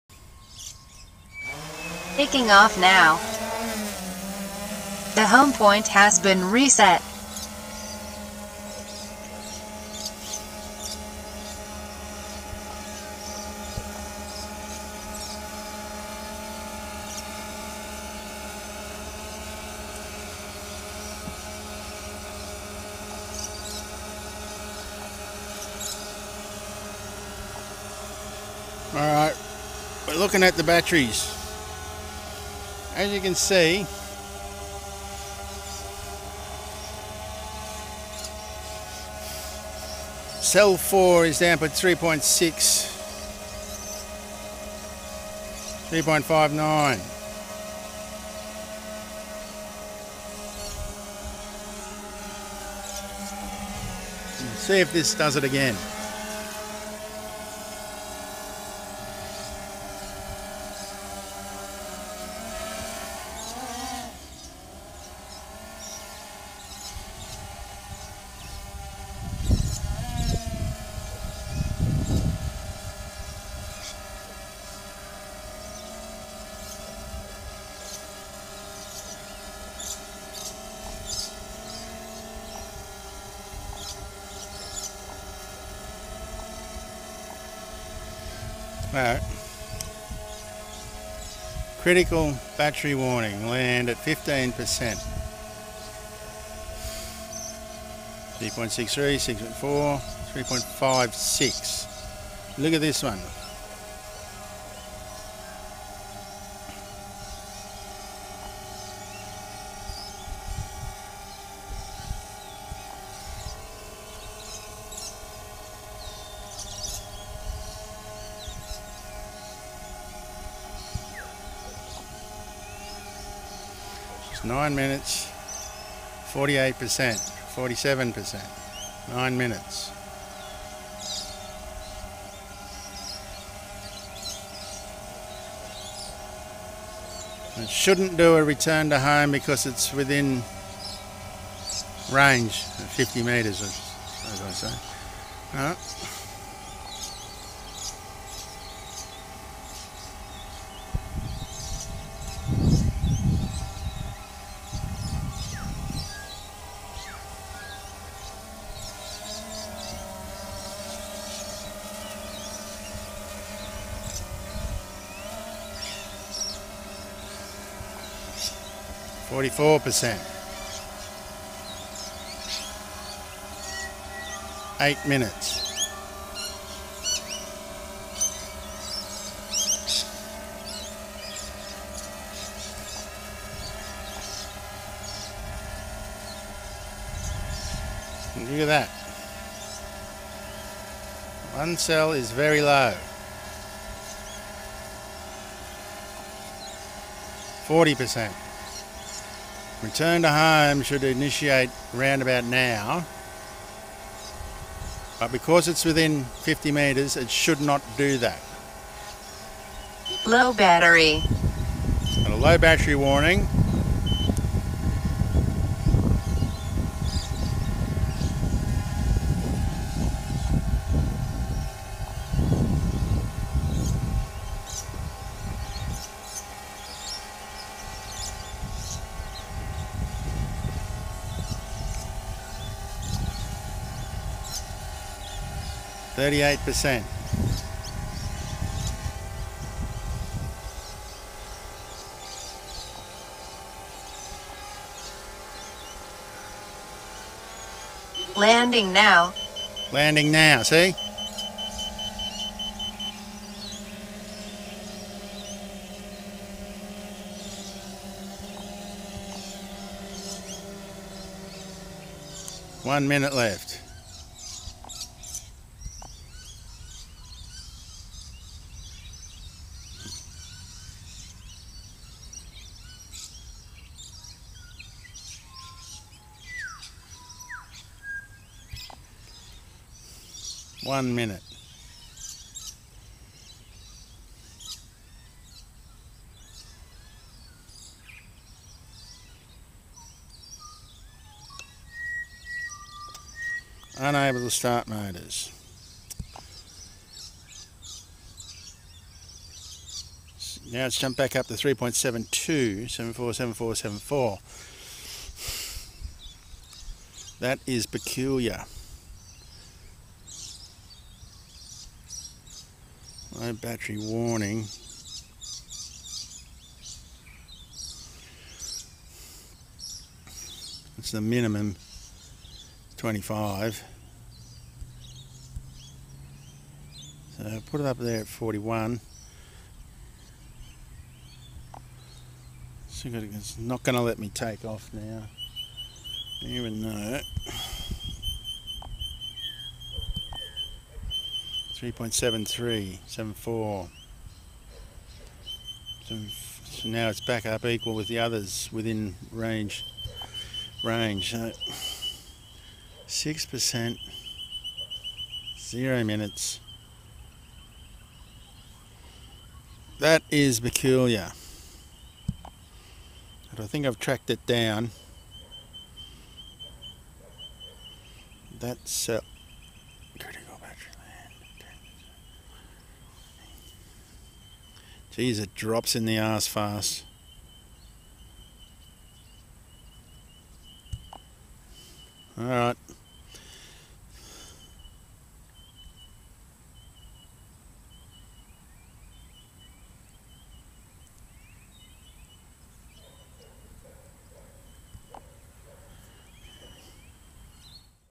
Screen Capture of the XStar Premiums dangerous Battery Faults
i bought 2 batteries when i bought my XSP, both have this fault, the first time it happened it went in the surf destroying it, i was able to recover it and contacted Autel and they were good enough to replace the complete craft and battery , now the original second battery is doing the same thing. and its only done around 38 charges , the same charges as the first one that failed, and says its about 85 percent healthy. whats going on anybody have any clues , i`m thinking of how to reset the batteries internal memory chip which could be causing the sudden drop in volts and then strangely it goes back to level with the others . this is worrying

Autel XStar Premium, NVidea Shield Tablet, Eachine GogglesTwo for FPV flight,